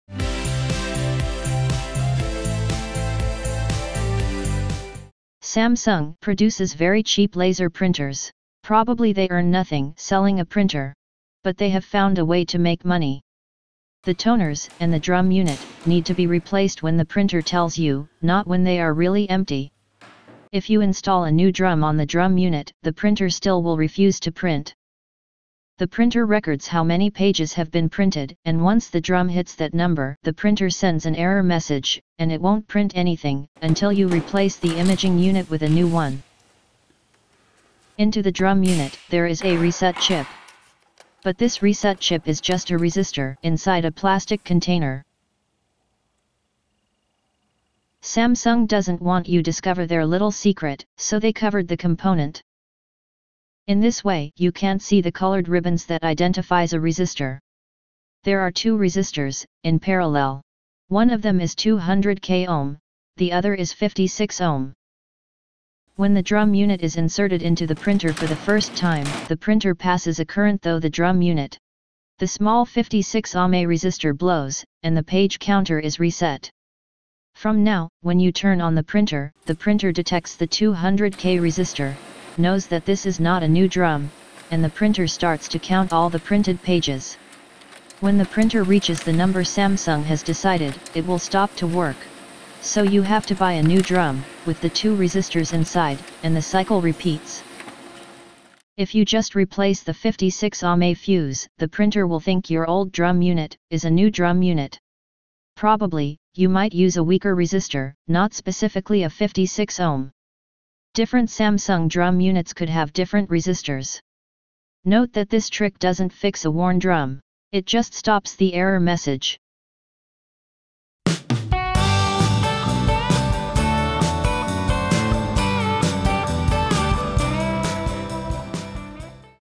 How the Samsung Reset chip really works
This video describes how the Samsung drum unit “chip“ works, and the method to reset a Samsung Drum Unit for laser printer.
- Activate subtitles in your language -